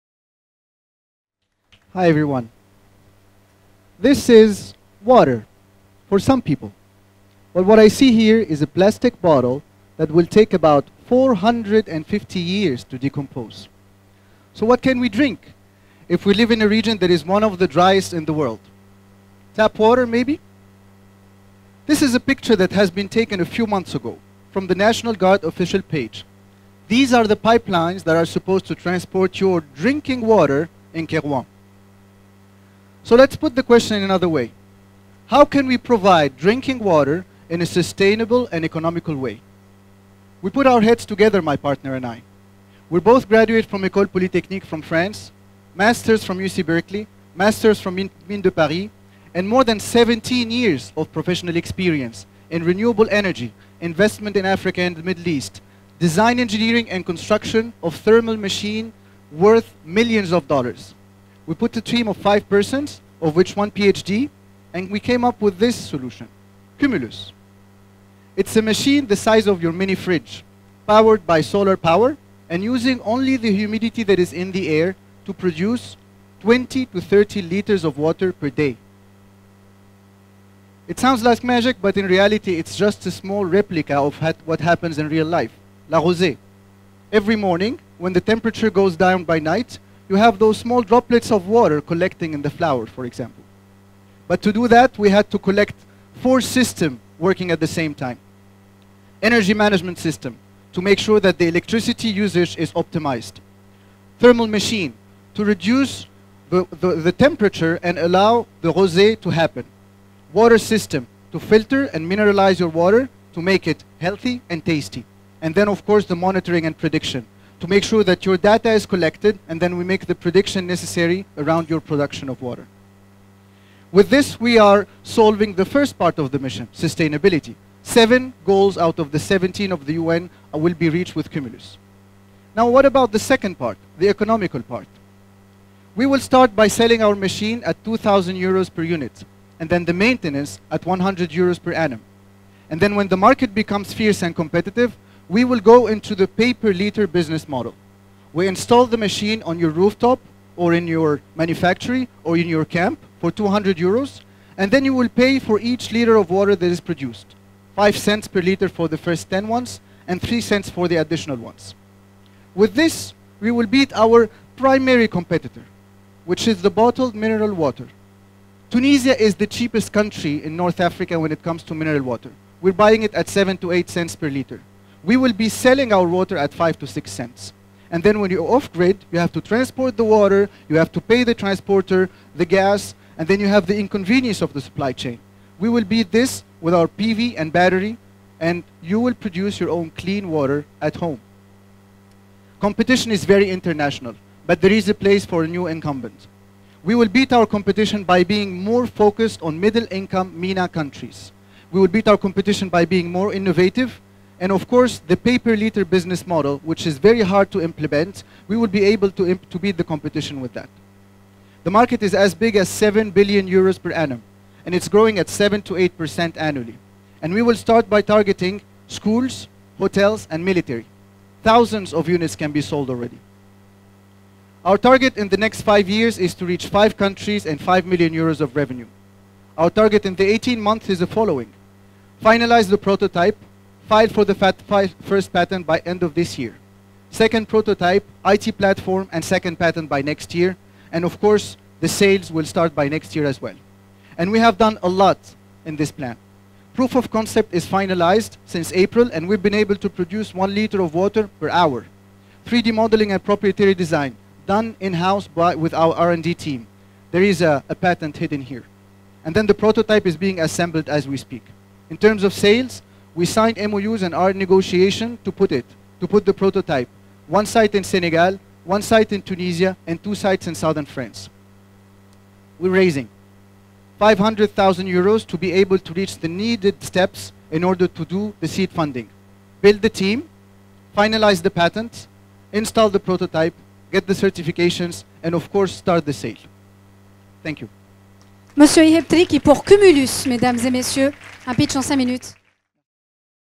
Kumululs - Orange Poesam - 1st price - Sept 2021
Kumulus is a tech start up in the water sector
in this video we speak about our technology, our market and our strategy